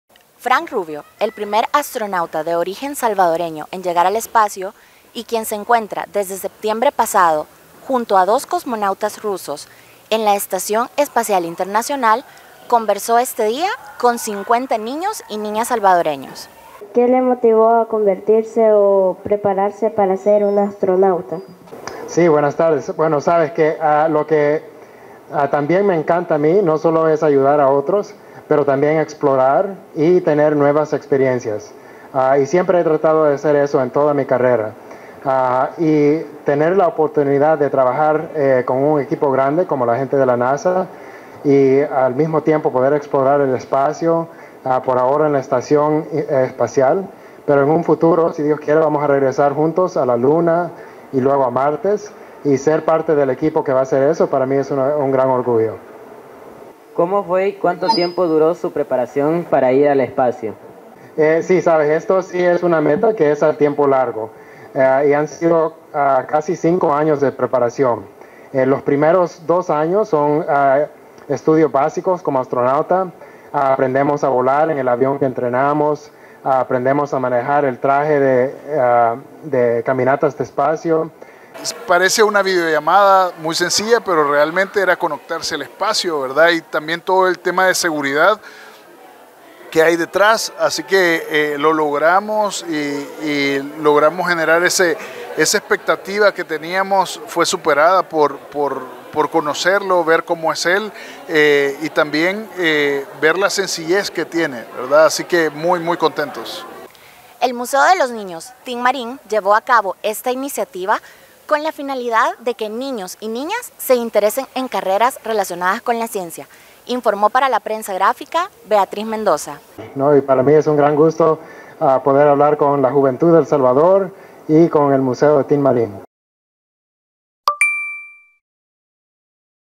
Astronauta Frank Rubio habla con niños salvadoreños
Frank Rubio, el primer astronauta de origen salvadoreño en llegar al espacio y quien se encuentra desde septiembre pasado en la Estación Espacial Internacional, conversó con 50 niños y niñas.